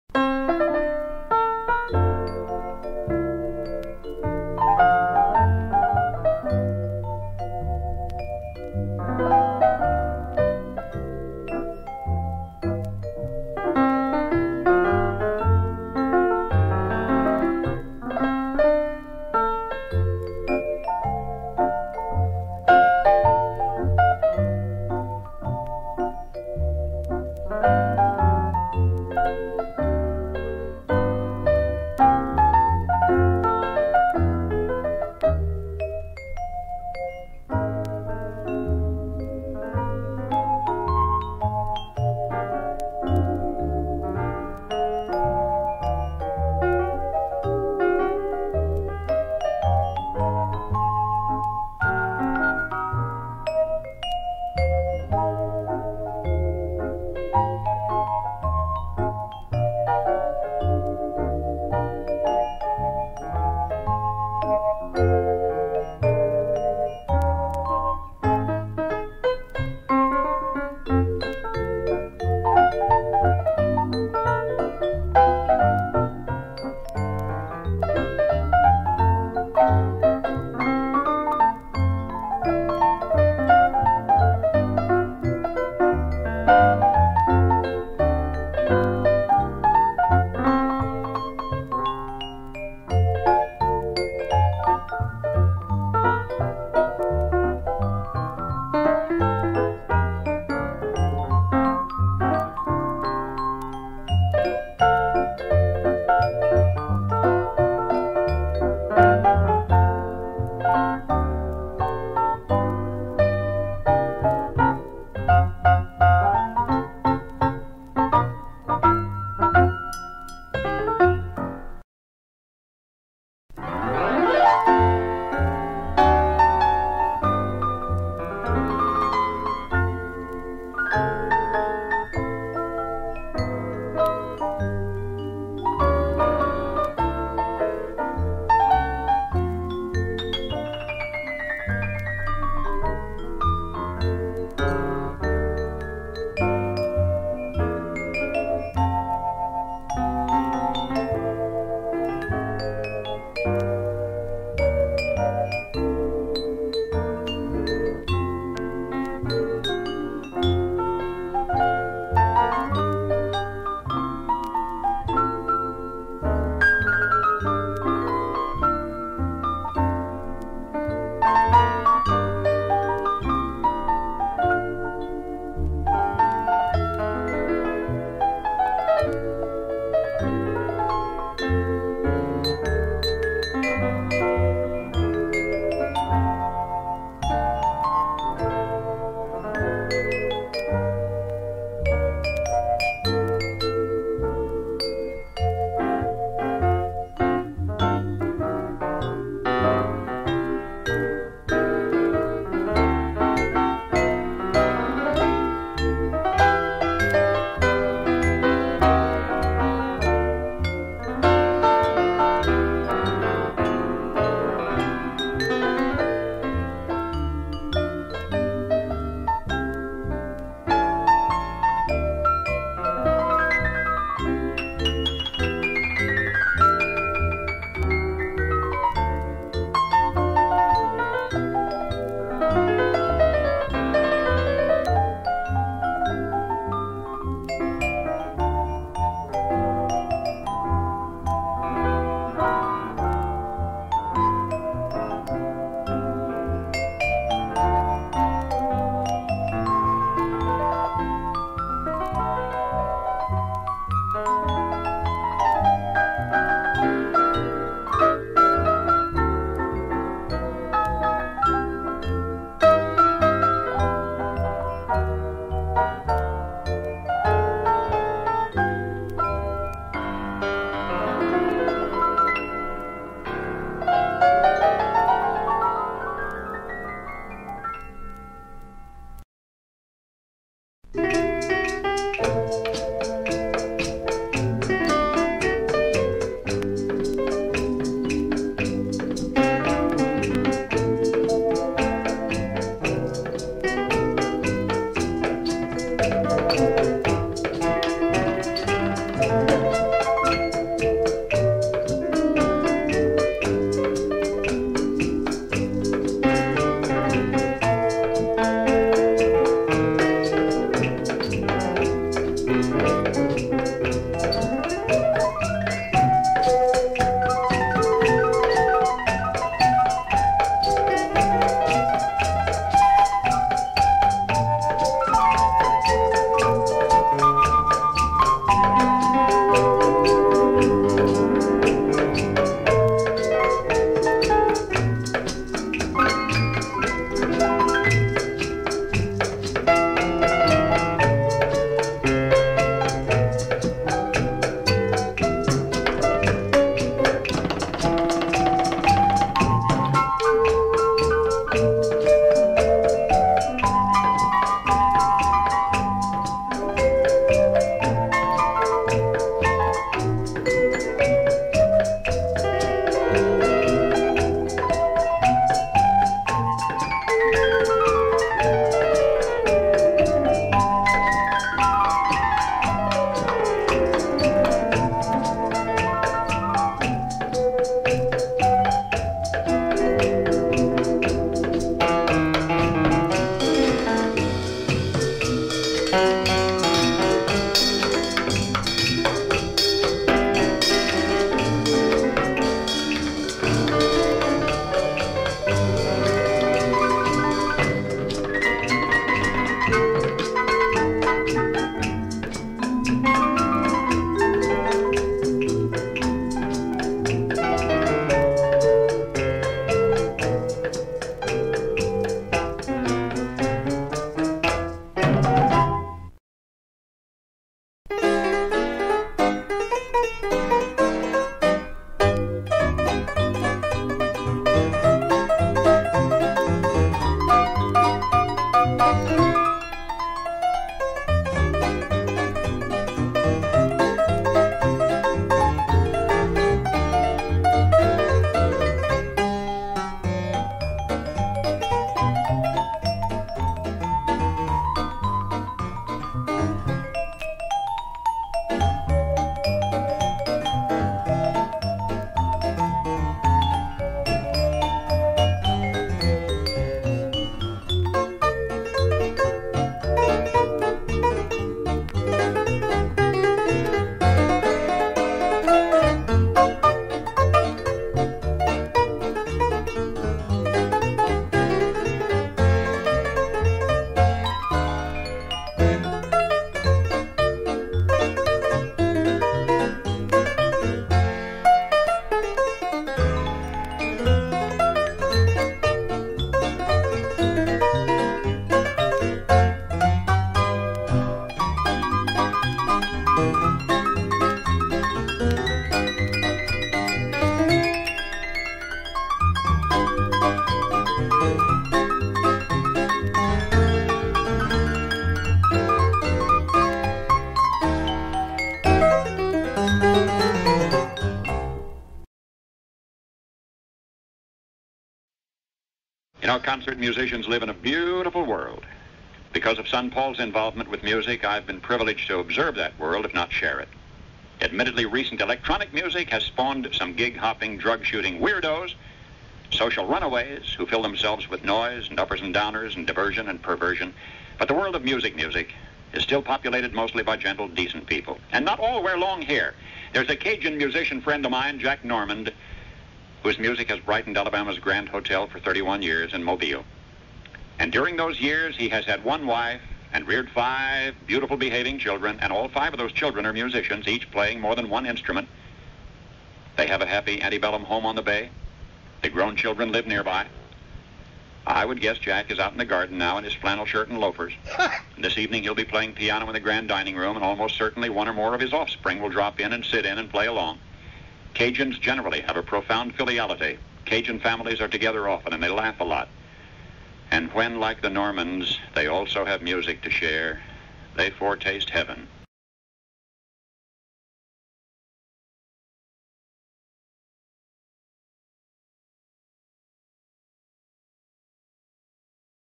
Jack Normand & Grand Hotel Orchestra by grandband.com
This is a short history of how Jack Normand came to be the house band leader at the prestigious (now Marriott) Grand Hotel in Point Clear, Alabama.  The Musician's Union told us they had never heard of any band playing 40 continuous years at one location.  Then after Jack died, the children continued the band for another ten years.  Coupled with the Grand opening start in 1941, the ending in 2000, the total time could be considered sixty years.  But it doesn't stop there.  In 1972, second son Glenn, fresh out of the Army, started his own group to handle the weddings and private party events in the area.  Now in its 46th year, his group, The Grand Band, has been involved in some 4,000 events in over 100 cities of 8 states, played for several governor's inaugural balls, and entertained the Kings and Queens of the Mobile Carnival Association for 40 years.